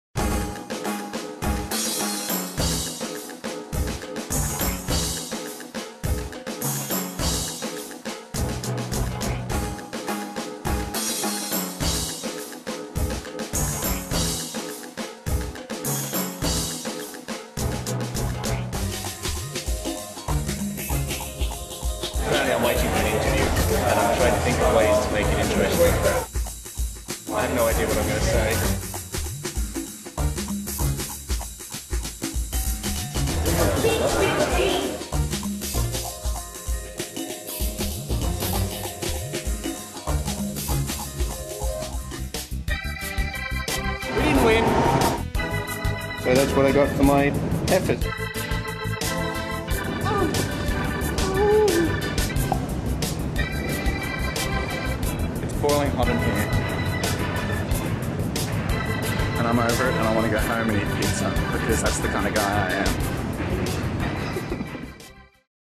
MTV/Optus 180 Australian Final
This is the full edit of the MTV/Optus final that appears in Episode 2 or our podcast - HOLLYWOOD or BUST! It was held in Sydney, and I was fortunate enough (goodness knows how) to make it into the top ten.